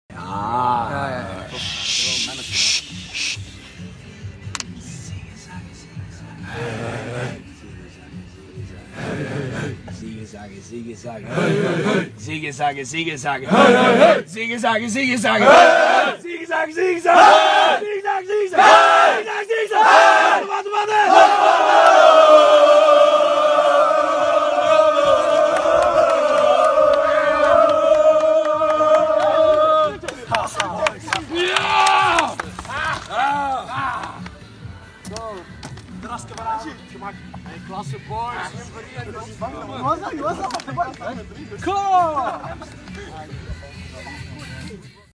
KFC Eppegem vs Avanti Stekene (2-5) [2018-04-08]
Mooi overwinning op KFC Eppegem, dat moet worden gevierd!
Gefilmd door Alain Van Mingeroet.